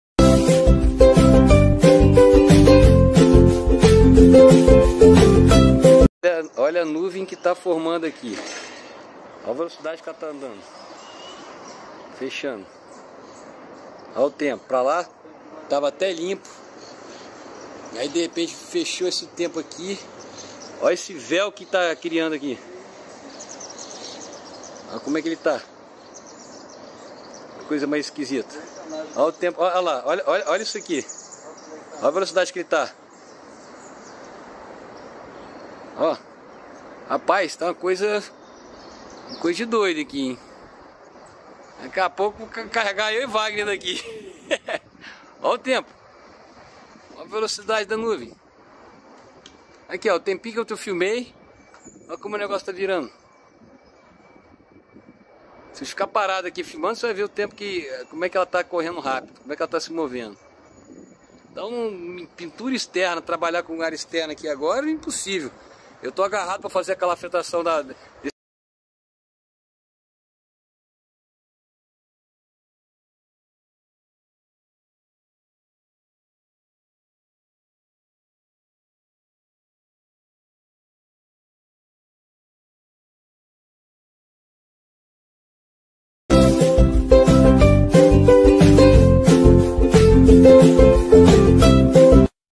Assustadora nuvem aparece em Guarapari no Espirito Santo.
Uma nuvem muito escura chamou atenção no céu de Guarapari na tarde desta quarta-feira dia 21/12/22, no vídeo é possível perceber que a formação se assemelha a uma espécie de tubo, o fenômeno é uma  nuvem-prateleira (shelf cloud, em inglês), tecnicamente conhecida como Cumulonimbus Arcus, que é associada a nuvem de tempestade.